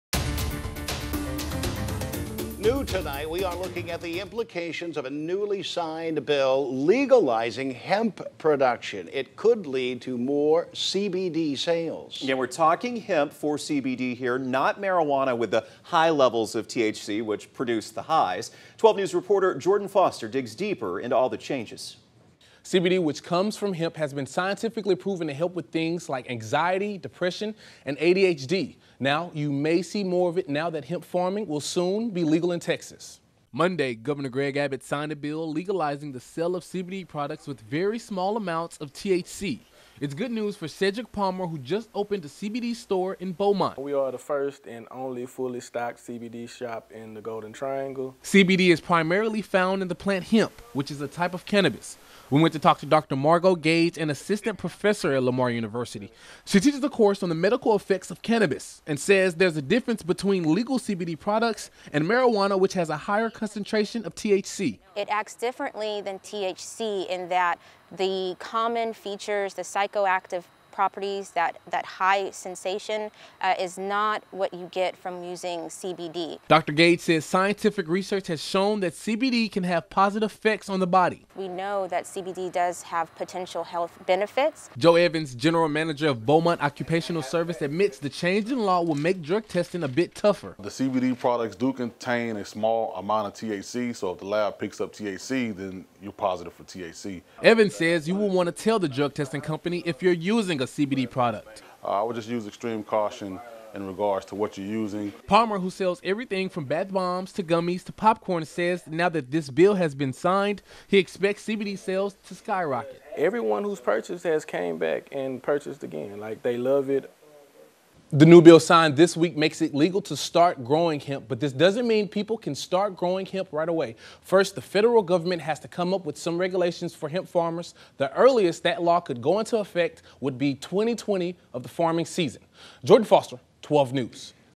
Beaumont business owner believes Texas bill legalizing hemp farming will boost CBD sales
An assistant professor at Lamar University said CBD can have potential health benefits.